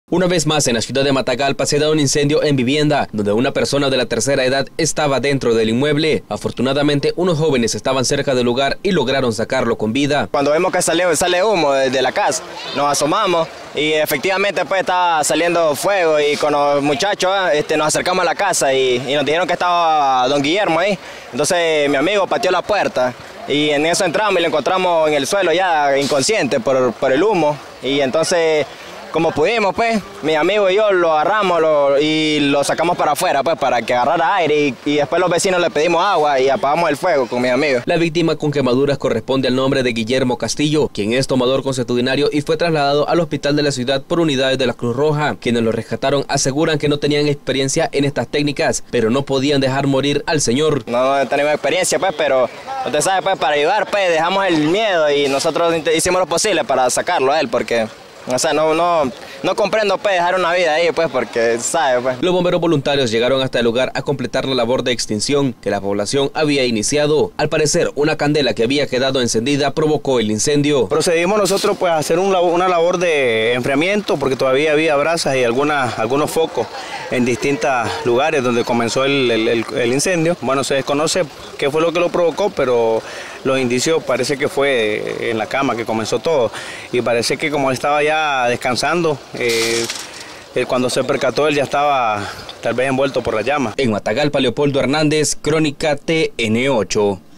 Una persona resulta con quemaduras al incendiarse su casa en Matagalpa - Nicaragua
La segunda noche en esta semana que en la ciudad de Matagalpa una casa arde en fuego, en esta ocasión fue en el barrio Lucidia Mantilla, en la parte sureste de la perla del septentrión.

Dentro del inmueble estaba una persona del sexo masculino, identificada como  Guillermo Castillo, quien resultó con quemaduras en el abdomen y brazos, por lo que tuvo que ser traslado por miembros de la Cruz Roja hacia el hospital escuela César Amador Molina.

La casa donde se dio el incendio es de concreto, con techo de madera y zinc, la persona que resultó quemada vivía sola, según testigos este es tomador consuetudinario y todas la noches al acostarse deja una candela a la orilla de la cama, lo que se presume fue la causa del incidente.

Frente a la casa está un Centro de Desarrollo Infantil (CDI), donde un grupo de jóvenes estaban reunidos para pasar la tarde, los que se dieron cuenta de que algo andaba mal dentro de la vivienda.

Cristóbal Sevilla, junto con otro grupos de jóvenes, fueron a ver, ya que identificaron la emisión de humo, al percatarse que en la parte interior habían llamas decidieron actuar.

“Uno de mis amigos le pegó una patada a la puerta, el humo estaba fuerte y miramos al señor tirado en el suelo inconsciente, por lo que decidimos entrar y rescatarlo, cuando los trajimos a la calle él ya tenía desprendida la piel en los brazos y abdomen, es cierto que no tenemos experiencia pero nos arriesgamos ya que no podíamos dejar al señor que se quemara”, dijo Sevilla.

Al lugar se hicieron presente los Bomberos Voluntarios, quienes al llegar lugar procedieron a hacer tareas de enfriamiento ya que la población había controlado las llamas, también llegó Policías Nacional, miembros de la Cruz Roja y líderes comunitarios, quienes trabajaron juntos para controlar la situación, hasta el momento el estado de salud de la persona quemada es estable.